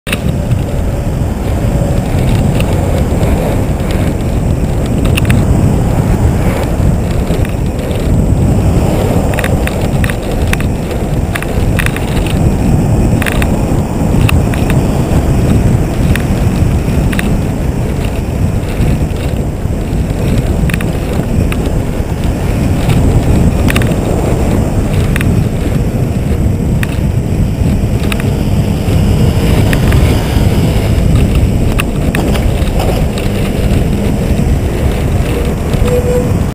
FY05VOJ MGIF ill-judged overtake and turn left
*Confused* Why would you drive like that? Hanging out on the wrong side of the road, just before a blind corner, and overtaking only to immediately turn left? Be patient, or you'll be a patient. More likely, you'll cause someone else to be a patient if you carry on driving like this for long enough.